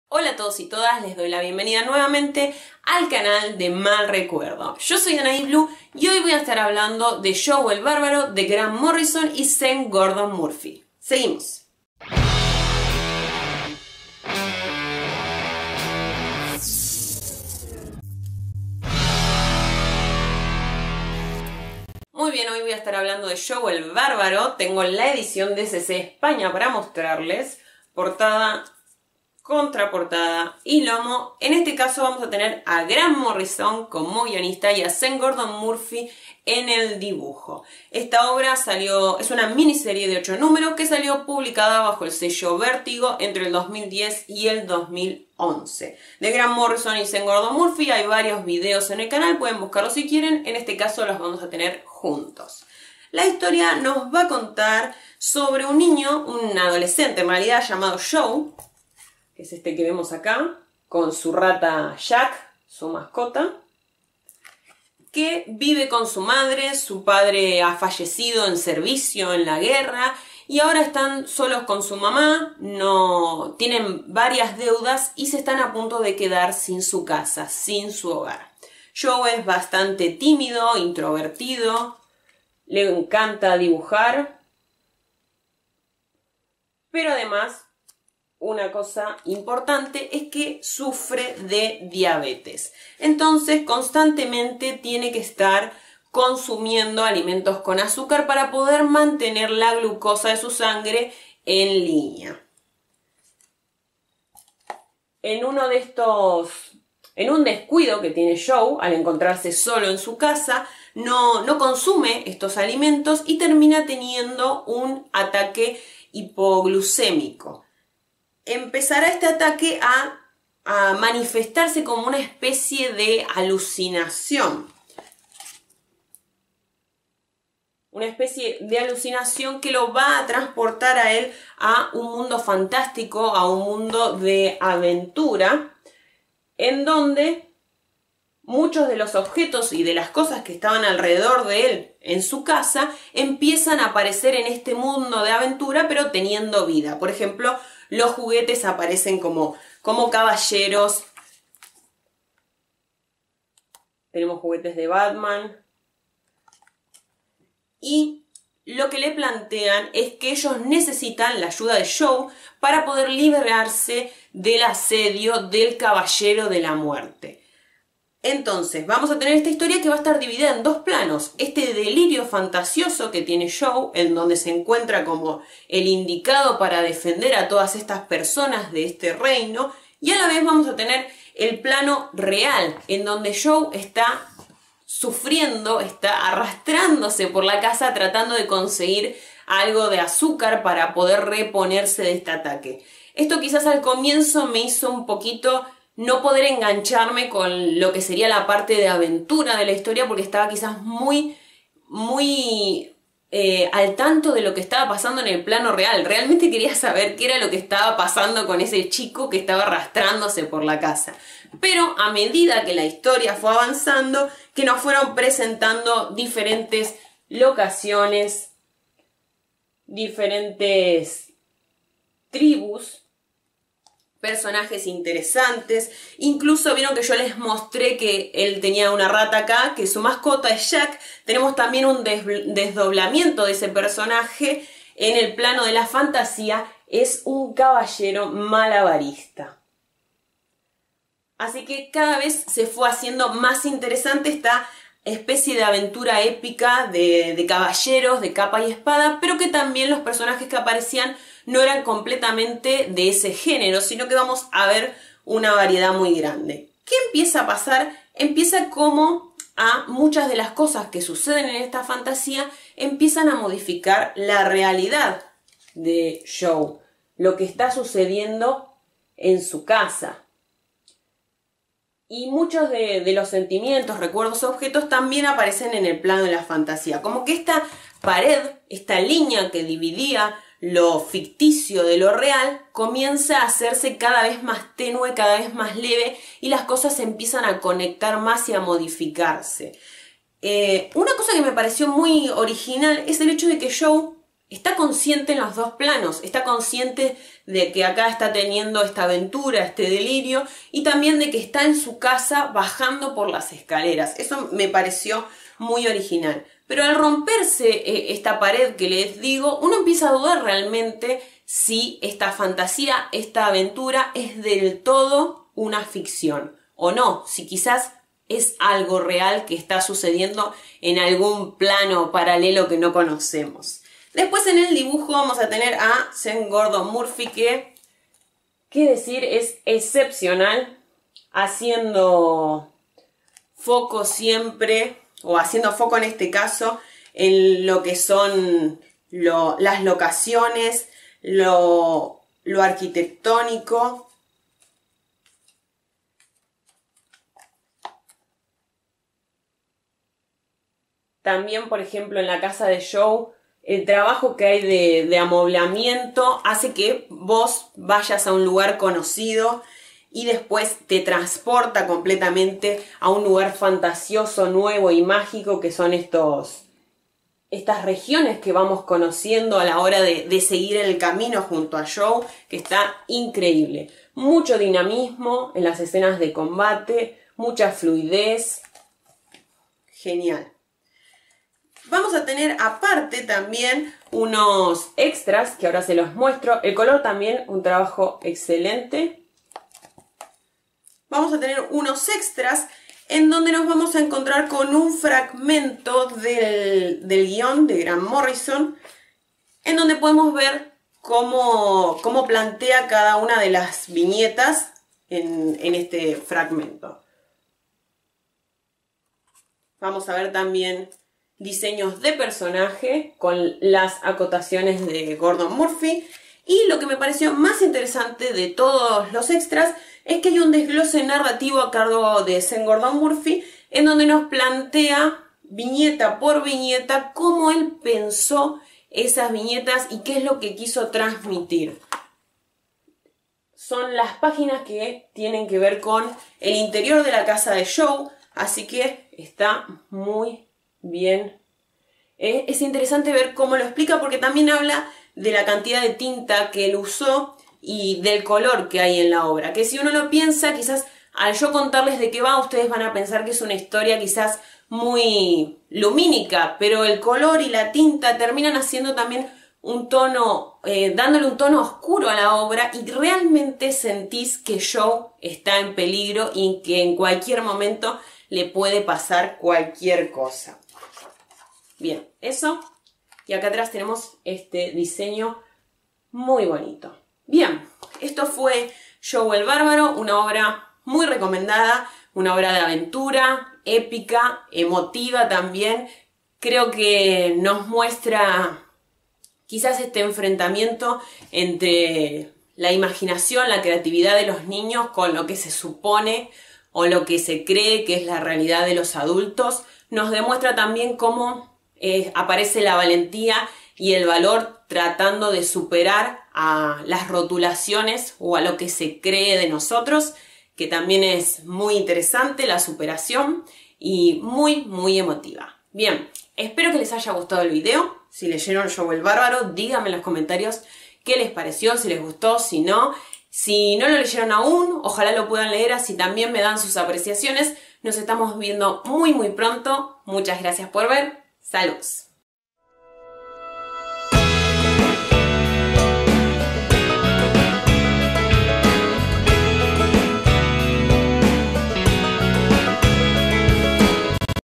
JOE EL BÁRBARO de Grant Morrison y Sean Murphy – Recomendación
Video reseña de JOE EL BÁRBARO guion de Grant Morrison, arte de Sean Gordon Murphy y colores de Dave Stewart .Editado en español por Ecc España. Formato cartone, 224 págs. A color, contiene extras!